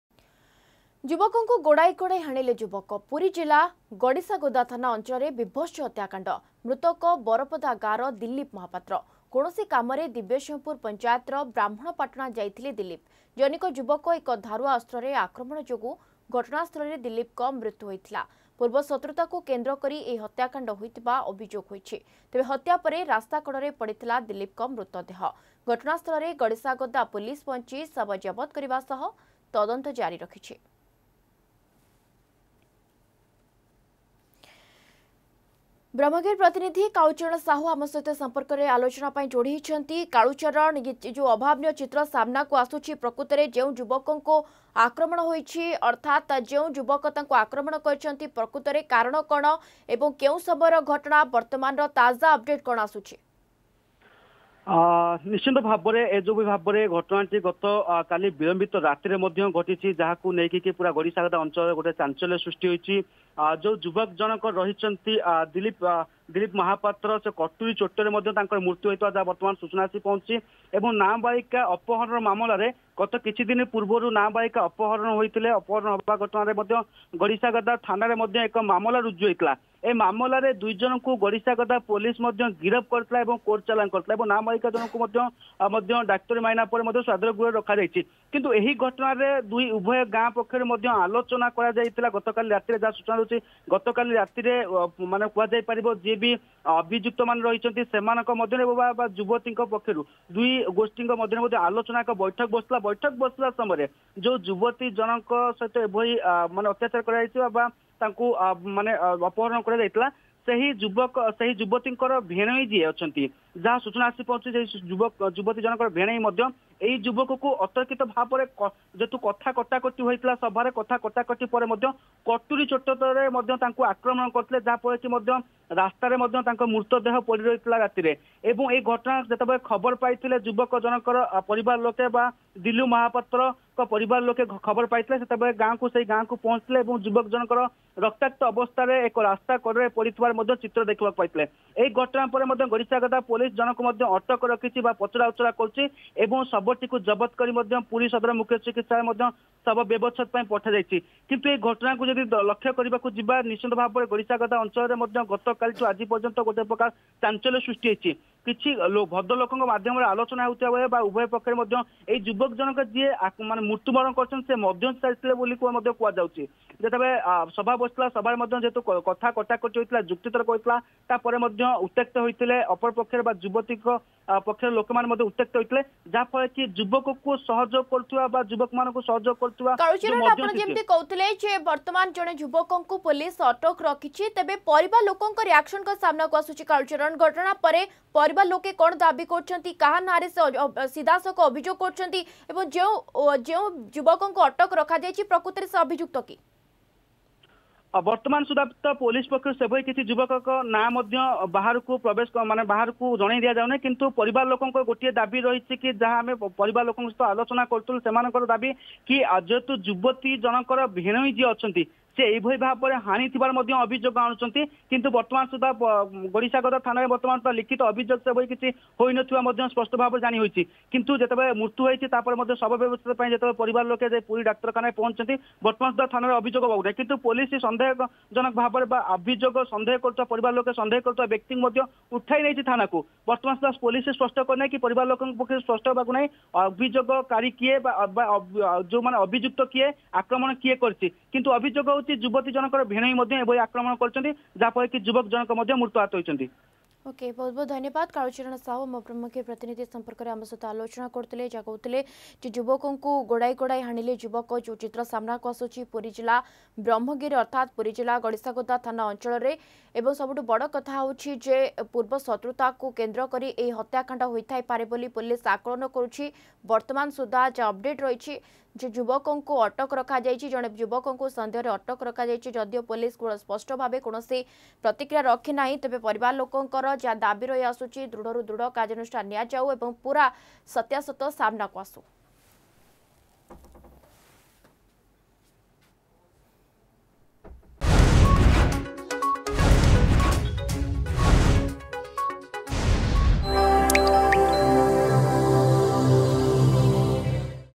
ଯୁବକଙ୍କୁ ଗୋଡାଇ ଗୋଡାଇ ହାଣିଲା ଯୁବକ ...
ଯୁବକଙ୍କୁ ଗୋଡାଇ ଗୋଡାଇ ହାଣିଲା ଯୁବକ । ପୁରୀ ଜିଲ୍ଲା ଗଡିଶାଗୋଦା ଥାନା ଅଞ୍ଚଳରେ ବିଭତ୍ସ ହତ୍ୟାକାଣ୍ଡ । ମୃତକ ବରପଦା ଗାଁର ଦିଲିପ୍ ମହାପାତ୍ର ...